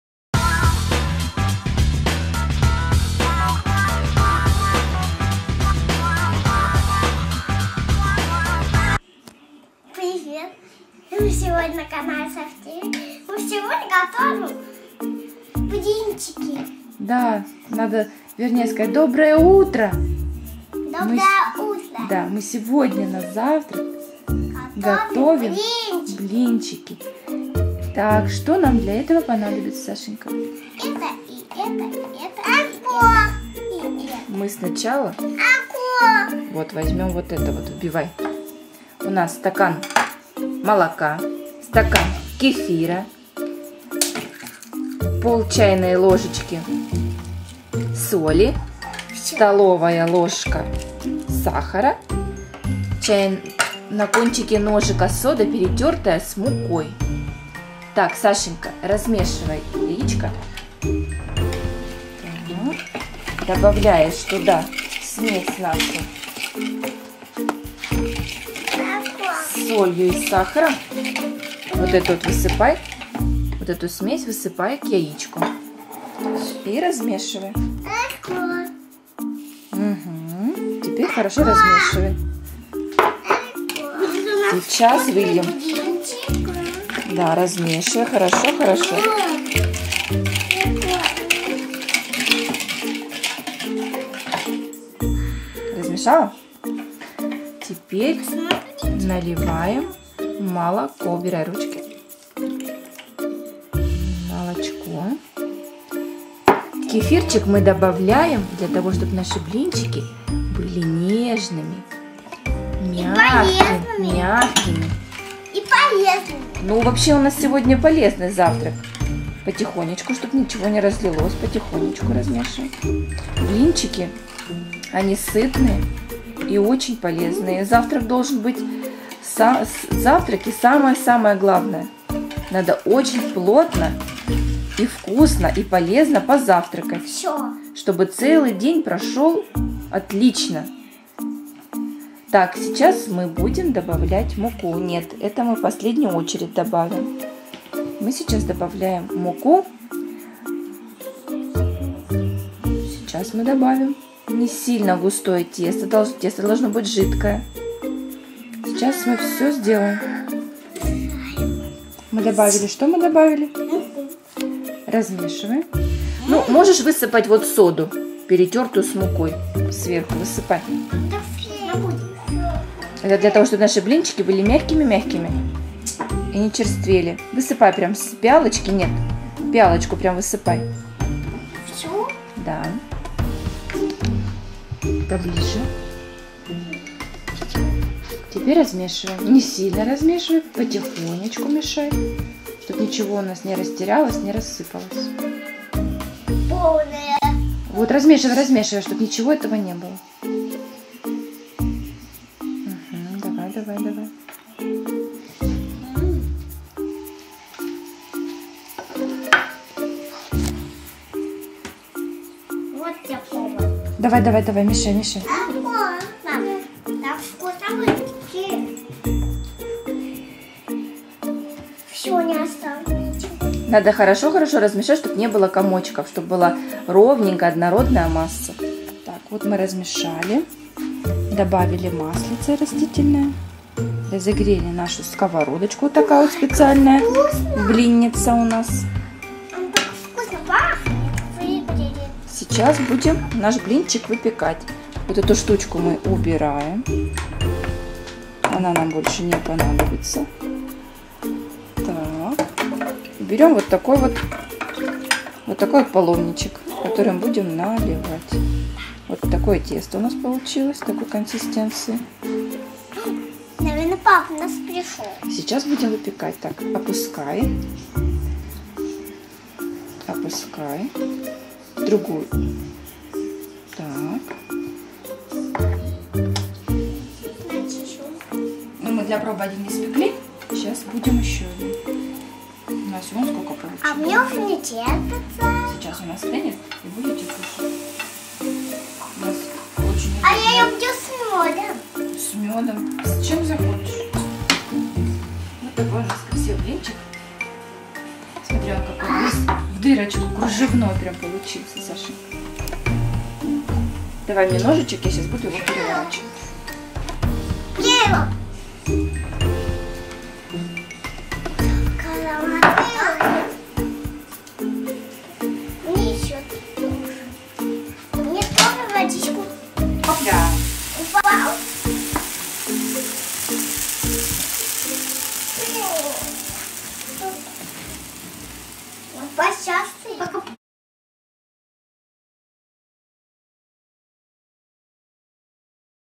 Блины на молоке | Тесто для блинов
Я с Сашей хочу поделиться с Вами моим рецептом очень тонких и вкусных домашних блинов (блинчиков).
Домашние Блины (Блинчики) Tasty Crepes получатся даже у тех, кто блины никогда не готовил.
Радуйте своих любимых и близких.
Приятного аппетита!
Как приготовить домашние блины & Домашние Блины (Блинчики) & Tasty Crepes Recipe?
Рецепт:
Сахар 1-2  столовые ложки
Мука 125 грамм
Молоко 250 грамм
Подсолнечное масло 2 столовые ложки